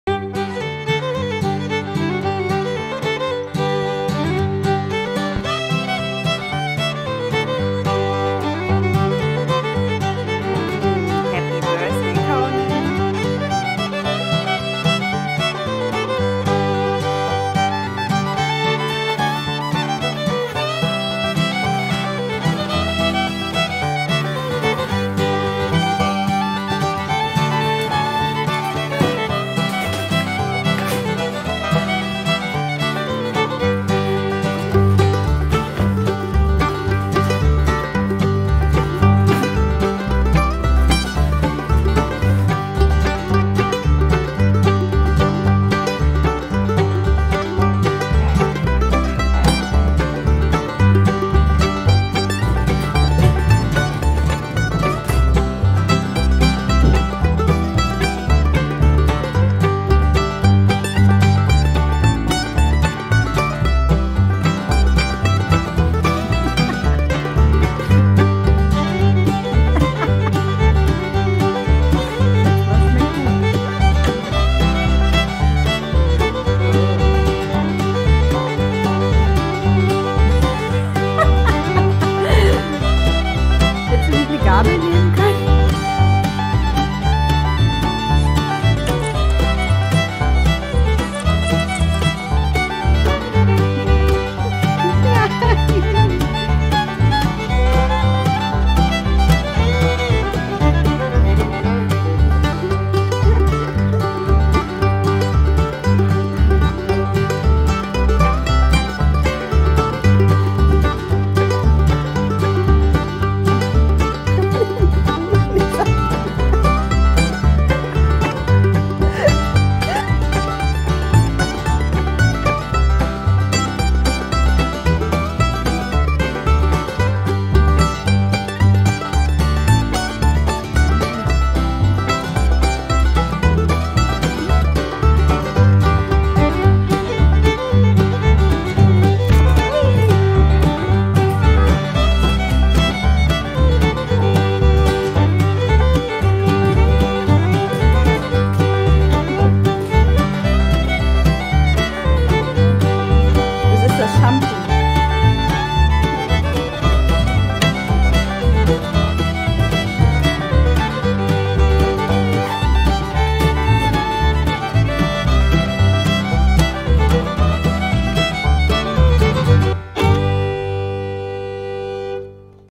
Conan, 11.-12. Lebensmonat
Conan feiert Geburtstag. Da darf eine Torte natürlich nicht fehlen.